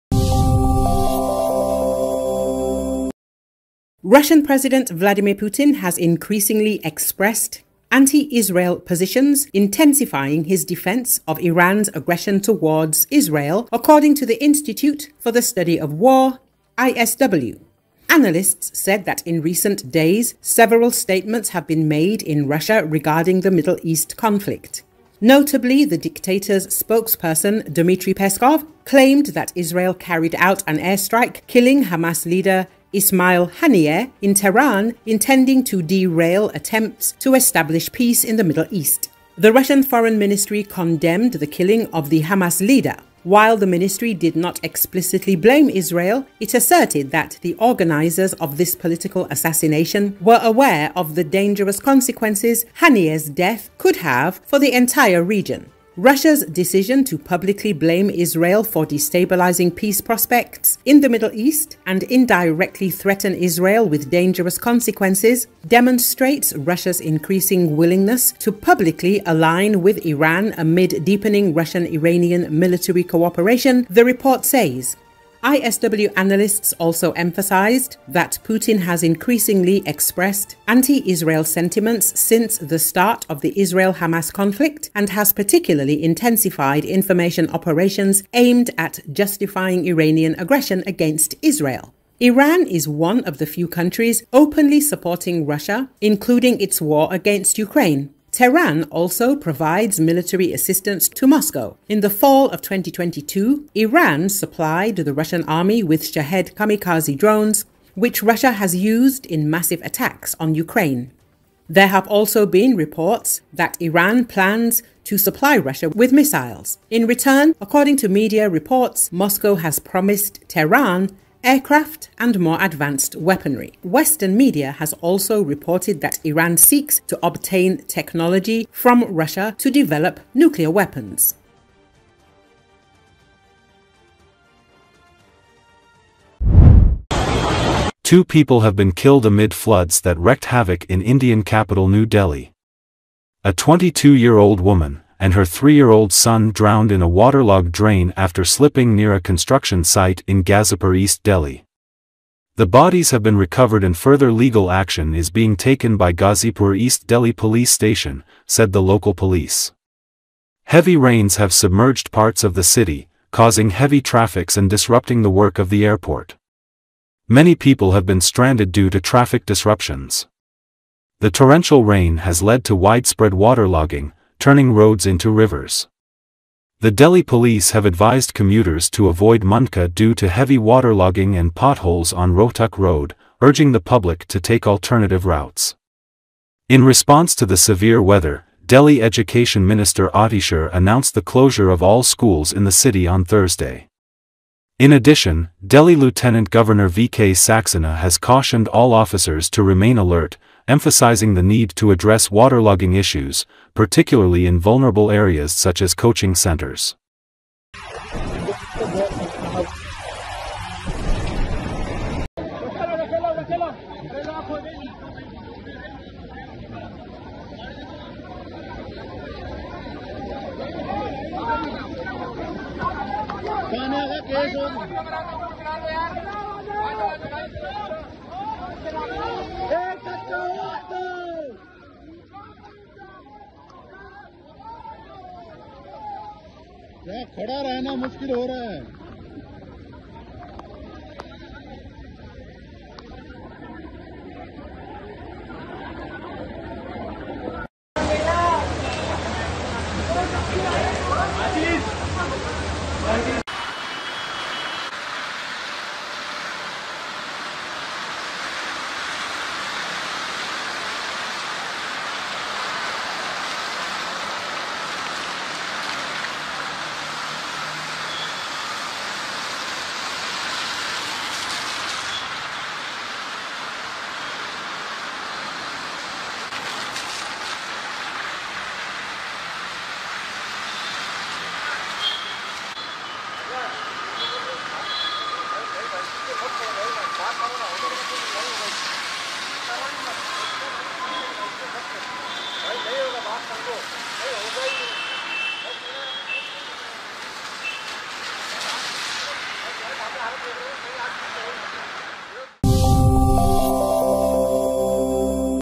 Putin intensifies justification of Iran's aggression against Israel – ISW
Russian president Vladimir Putin has increasingly expressed anti-Israel positions, intensifying his defense of Iran’s aggression towards Israel, according to the Institute for the Study of War (ISW).

Analysts said that in recent days, several statements have been made in Russia regarding the Middle East conflict. Notably, the dictator’s spokesperson, Dmitry Peskov, claimed that Israel carried out an airstrike, killing Hamas leader Ismail Haniyeh in Tehran, intending to "derail attempts to establish peace in the Middle East."

The Russian Foreign Ministry condemned the killing of the Hamas leader. While the Ministry did not explicitly blame Israel, it asserted that the "organizers of this political assassination" were aware of the "dangerous consequences" Haniyeh’s death could have for the entire region.

"Russia's decision to publicly blame Israel for destabilizing peace prospects in the Middle East and indirectly threaten Israel with "dangerous consequences" demonstrates Russia's increasing willingness to publicly align with Iran amid deepening Russian-Iranian military cooperation," the report says.

ISW analysts also emphasized that Putin has increasingly expressed anti-Israel sentiments since the start of the Israel-Hamas conflict, and has particularly intensified information operations aimed at justifying Iranian aggression against Israel.

Iran is one of the few countries openly supporting Russia, including its war against Ukraine. Tehran also provides military assistance to Moscow. In the fall of 2022, Iran supplied the Russian army with Shahed kamikaze drones, which Russia has used in massive attacks on Ukraine.

There have also been reports that Iran plans to supply Russia with missiles. In return, according to media reports, Moscow has promised Tehran aircraft and more advanced weaponry.

Western media have also reported that Iran seeks to obtain technology from Russia to develop nuclear weapons.